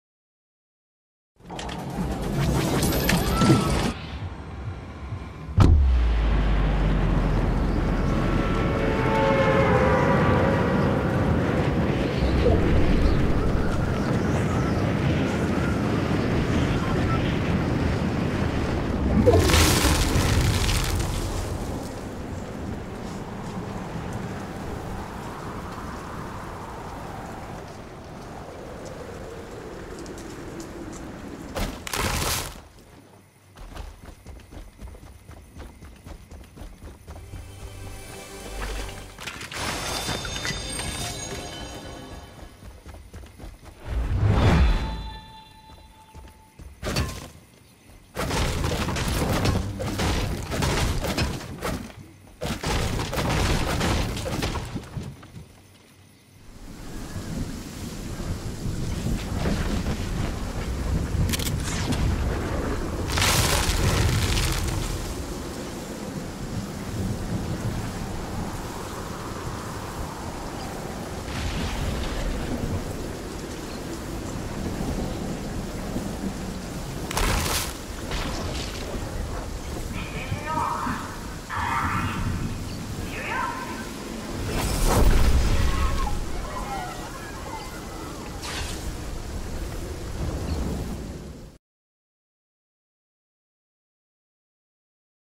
New Secret Gnome Challenge
In this video I show you how to get some bonus xp by visiting these two locations.